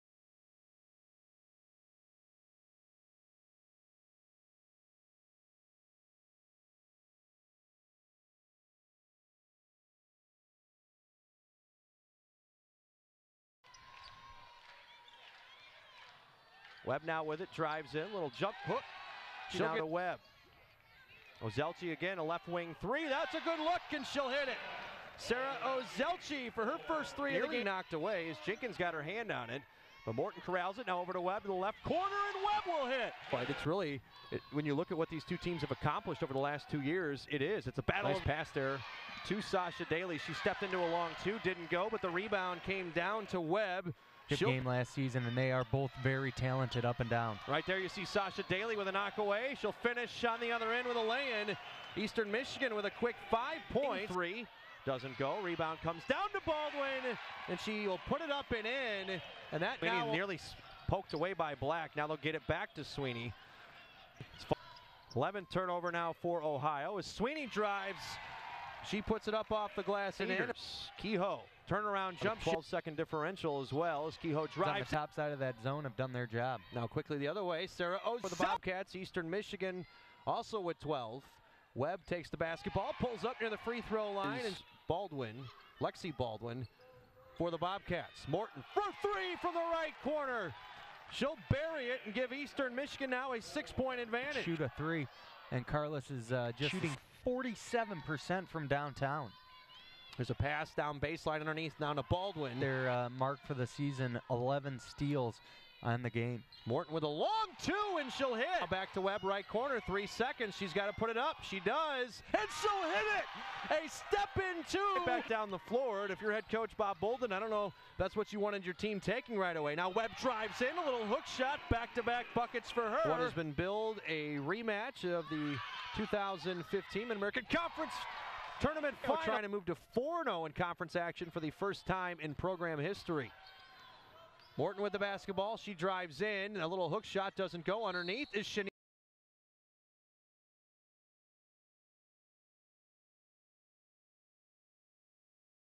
Eagles Fall in Rematch of 2015 MAC Championship Game
In a rematch of the 2015 Mid-American Conference Championship game, the Eastern Michigan University women's basketball team fell at home, 71-64, against Ohio University. Individually, junior Phillis Webb (Brooklyn Park, Minn. – Osseo) and graduate transfer Chanise Baldwin (Pontiac, Mich. – Martin Luther King) led the Eagles (11-4, 2-2 MAC), posting double-doubles on the night.